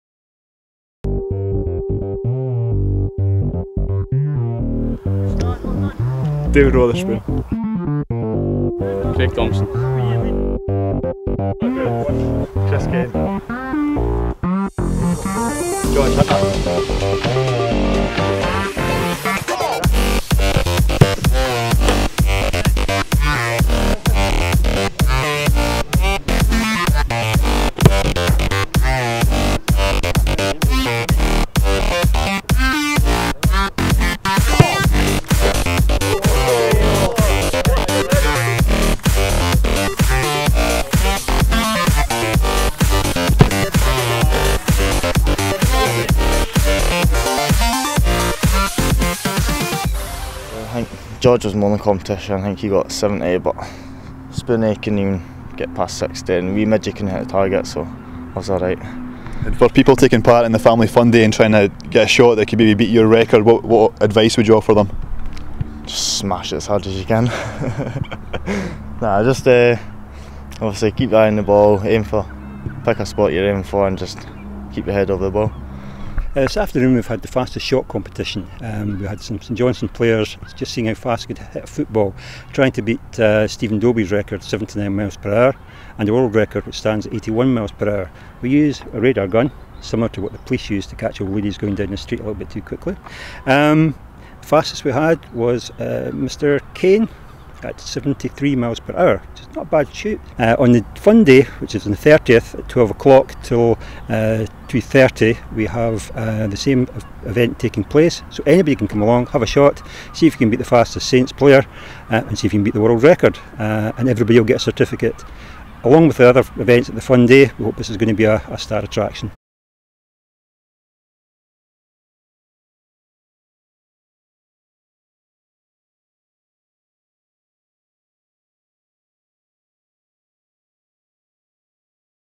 St. Johnstone FC - "who has the hardest shot?"
David Wotherspoon, Craig Thomson, Chris Kane and George Hunter take each other on to see who has the most powerful deadball shot.

Saints fans can try to beat the winner's record at the Family Fun Day on Saturday 30th July at McDiarmid Park.